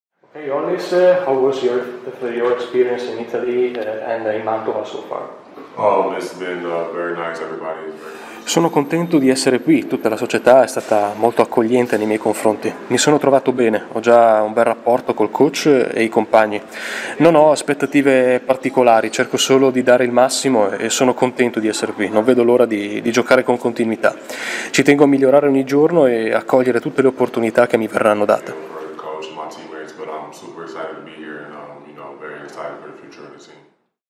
Hollis Thompson, la stella degli Sting si racconta in Gazzetta
L'americano classe 1991: «Ho iniziato a giocare a basket dall'infanzia - attacca Thompson -. Quando guardavo i campioni dei Lakers, la mia squadra preferita, in tv, avevo già capito cosa avrei voluto fare nella vita. Anche senza aver avuto genitori o parenti sportivi, sono cresciuto in una famiglia di grandi appassionati, in cui mio padre mi ha insegnato qualche trucco per realizzare i primi canestri».